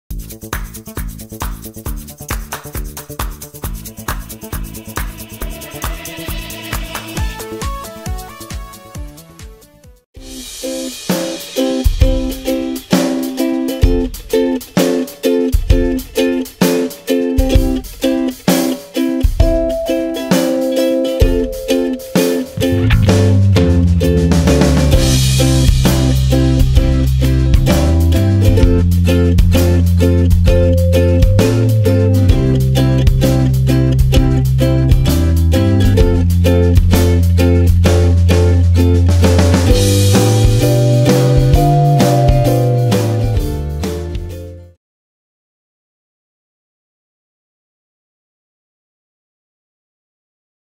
J3 Posterior Backrest With TH Hardware - Sunrise Medical Australia
A guide to our J3 Posterior Backrest and how to install and mount the TH Hardware.